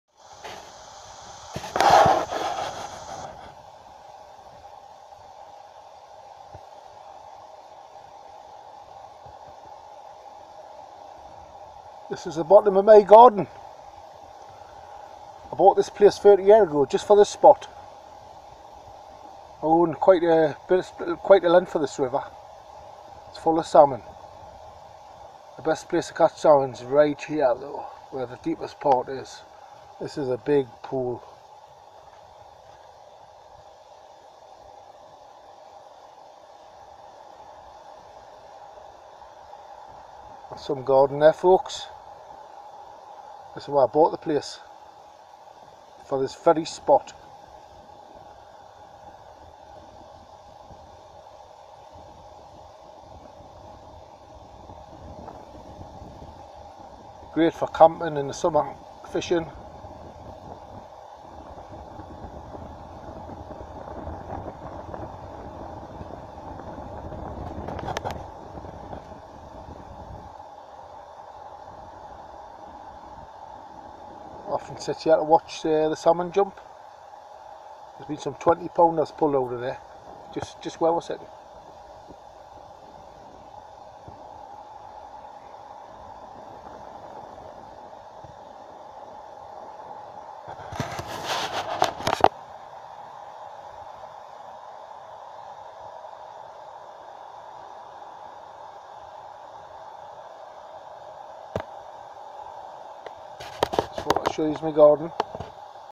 The bottom of my garden where very large salmon jump quite often.
This is part of my garden where it is great for camping and fishing salmon\trout this river here which I own quite a good stretch of has won the best fished salmon river in whole of the UK for 3 years running now.